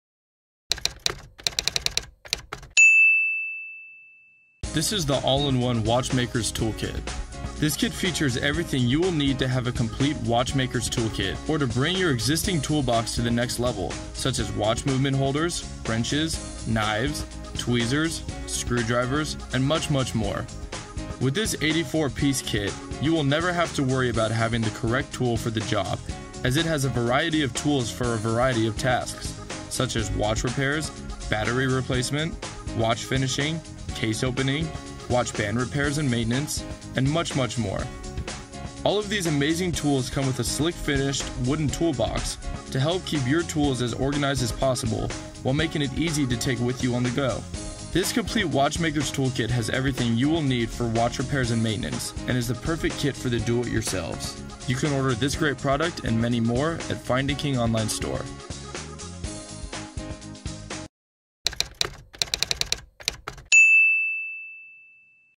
Watch Maker's Tool Kit
This is a new set of 64 watch repair tools. The box measures approximately 16 1/2" x 10 3/4" x 3 1/8" (41.8 cm x 27.3 cm x 79 mm).

You receive:

One crystal lift

One case wrench with 4 sets of pins

3 Brushes for greasing and cleaning

4 Movement holders

One dust blower

One 3 in 1 oil cup

2 Oil pins

One pair of pliers for punching holes

One pair of notching pliers

One Benzene cup with lid

One triangular sorting tray

2 Screwdrivers flat head

6 Assorted files

5 Tweezers

One pin vise with 2 collets

2 Polishing cloths that measure approximately 12" x 12" (30.4 X 30.4 cm)

2 Spring bar removal tools

One oil stone

24 Wood peg bundle

One case opener knife

One riveting stake with 36 holes

One Presto style hand remover

One tube of adhesive